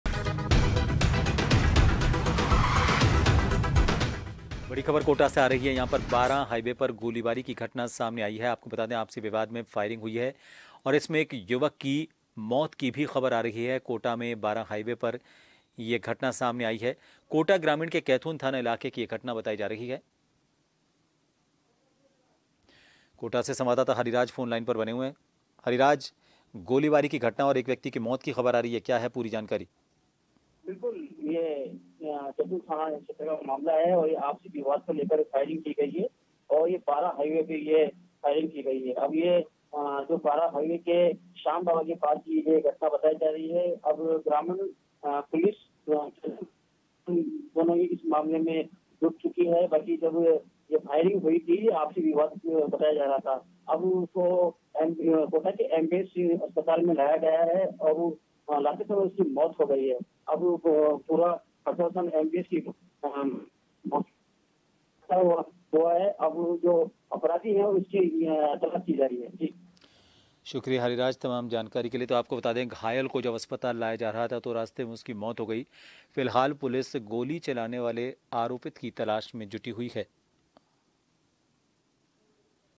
Kota- आपसी विवाद में फायरिंग में युवक की मौत | jantv
Watch JAN TV on :
Tata Play DTH : 1185
Airtel DTH: 355
JIO Fiber: 1384